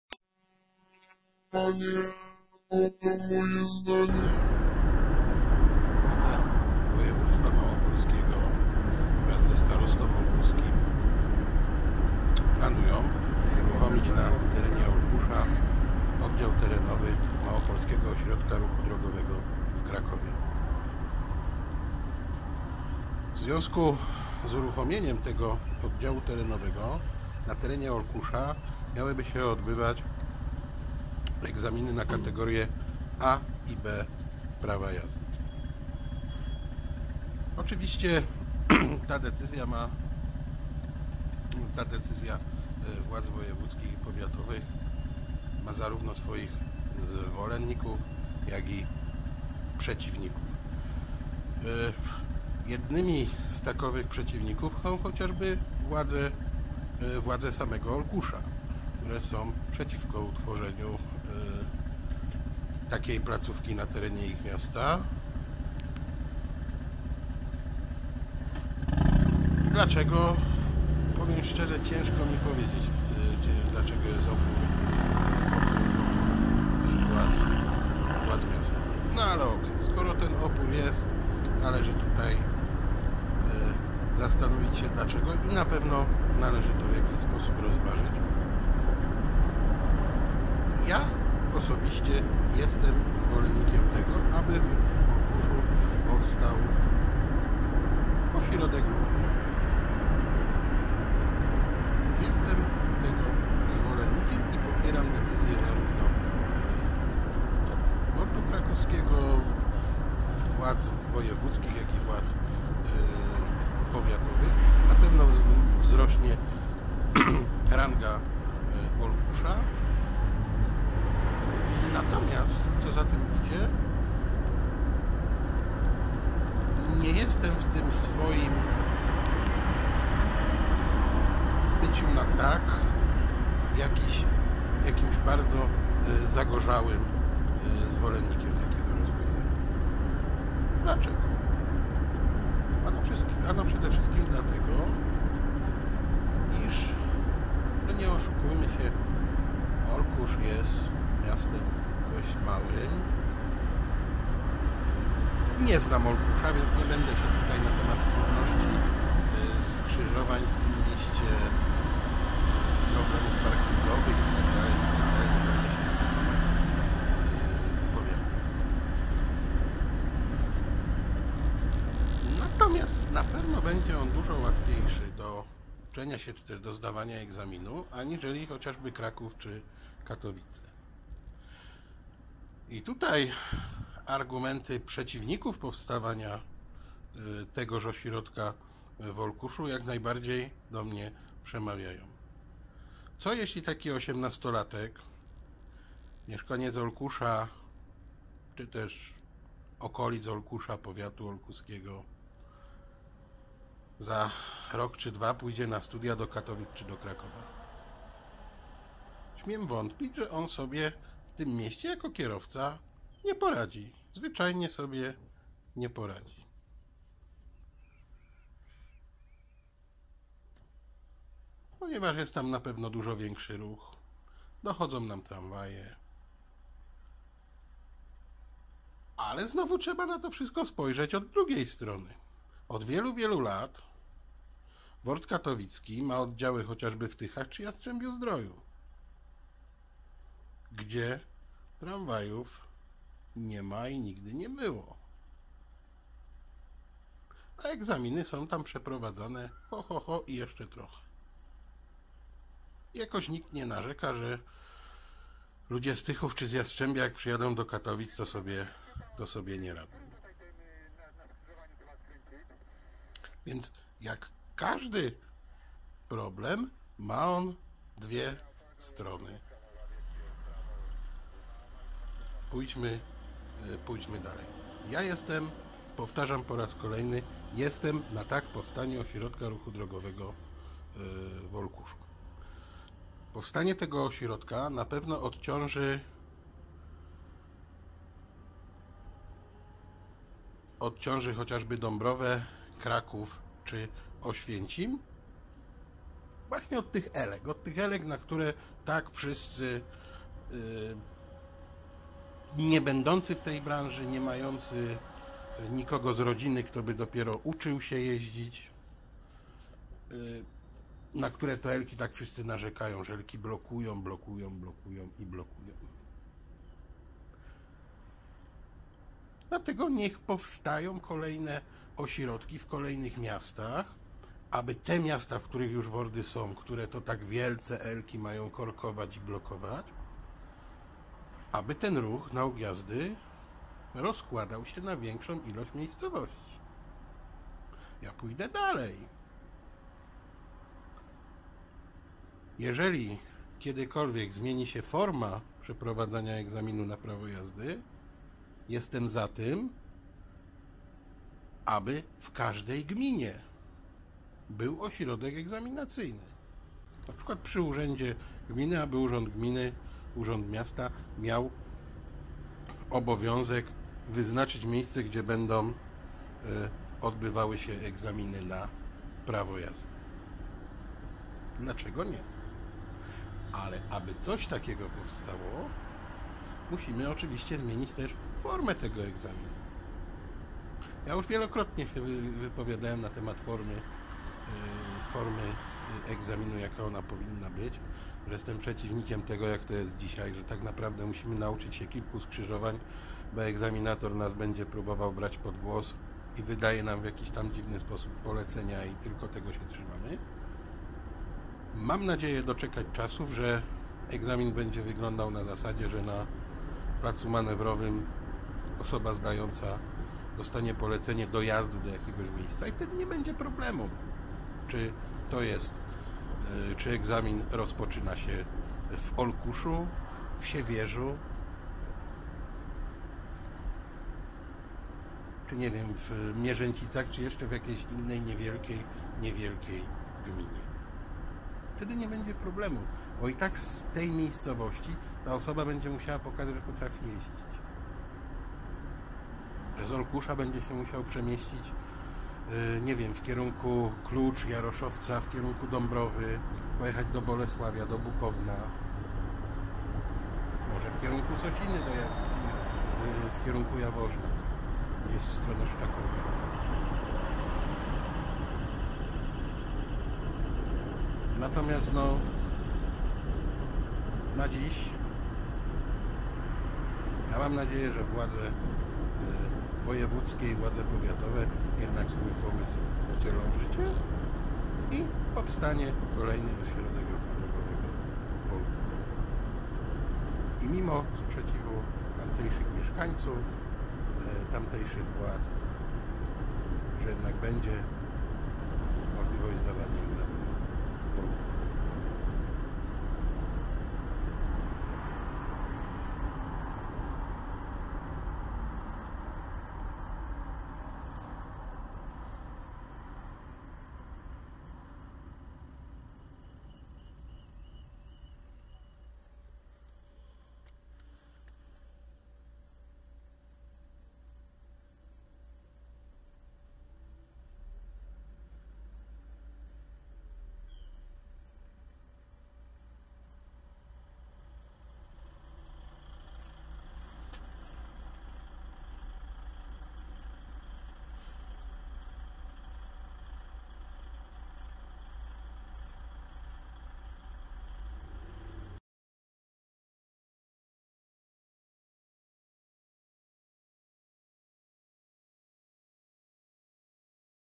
Moim zdaniem... Egzaminy w Olkuszu
Po wakacyjnej przerwie wracam z rubryką "moim zdaniem". Na początek coś o planowanym utworzeniu w Olkuszu oddział terenowego MORD Kraków. Ja jestem na TAK! Co nie oznacza, że nie dostrzegam minusów takiego rozwiązania...